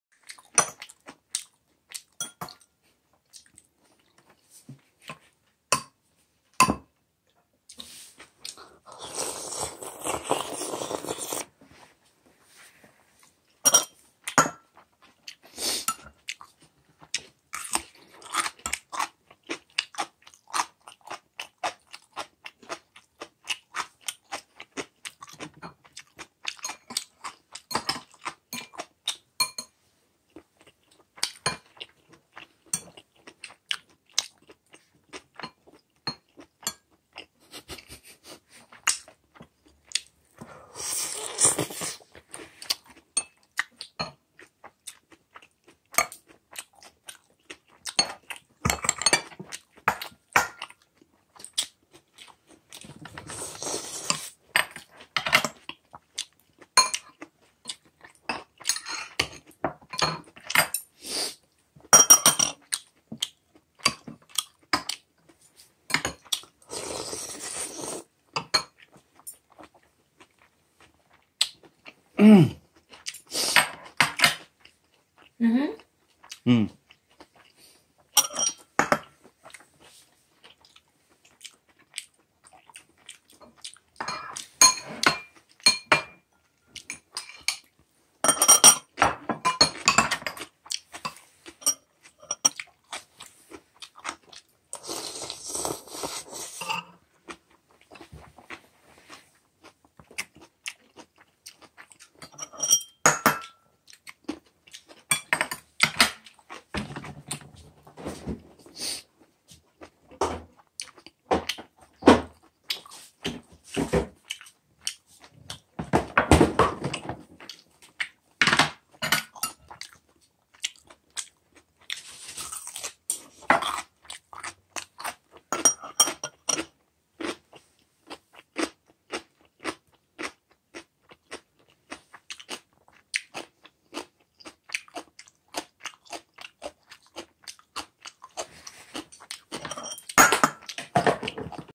[먹방] ASMR prosessing to make poop
삼겹살. 곰탕. 깍뚜기. 밥. 샐러드가 똥이 되어가는 첫번째 과정을 과감하게 표현. 외로울땐? 구독버튼